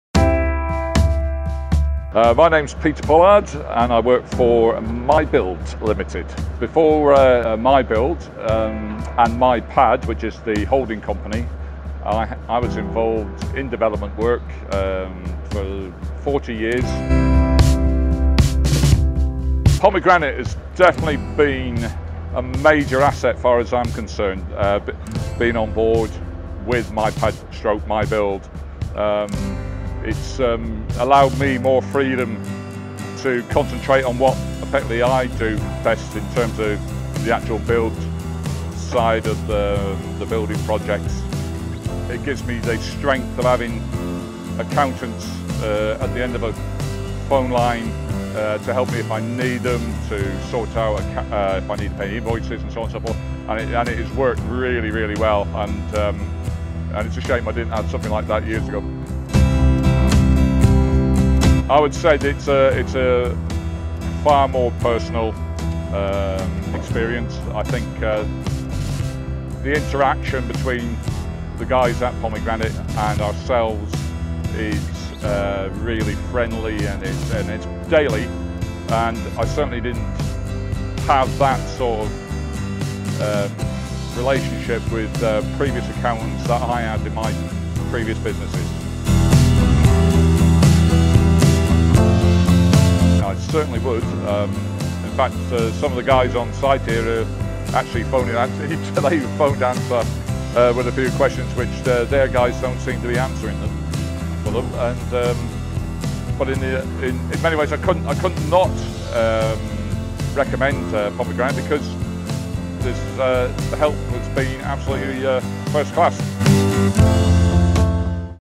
My Build (NW) Limited
House Builder Pete Pollard discuss the benefits he has found for his company from working with Pomegranate Consulting